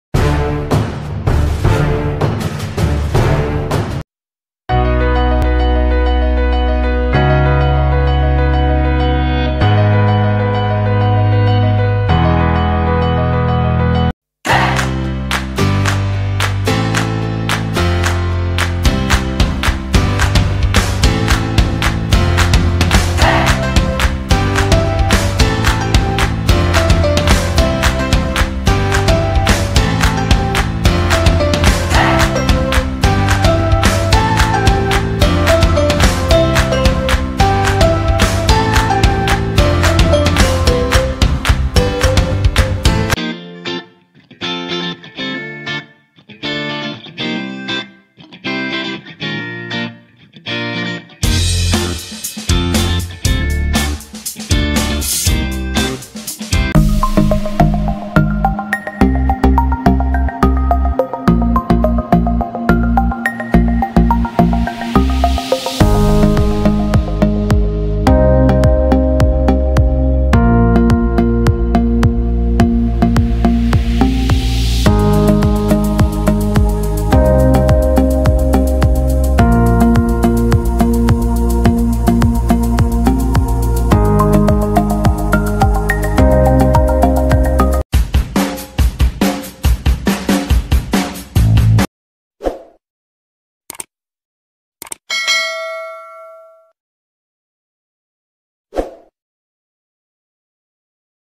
MAGPALIIT TAYO NG ABS 🤪😂 | EXERCISE | KaSafetyKo
Medyo hindi na proportion ang curve kaya need na paliitin ang abs 🤪😂🤣.

Kahit sa bahay lang or opisina basta't willing ka lang mag exercise kaya naman pala 😂.